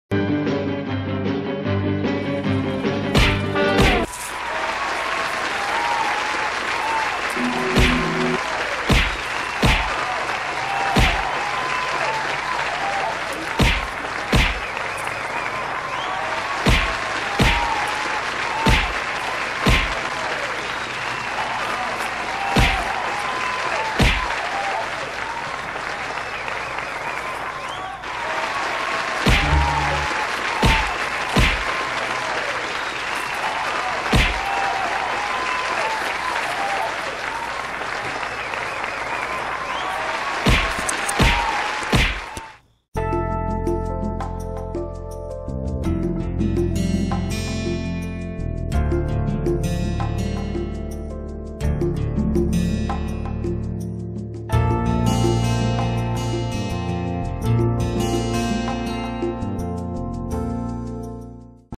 Kittens Fighting MMA Style
Kittens Fighting MMA Style. What could be cuter than two little kitties fighting MMA style?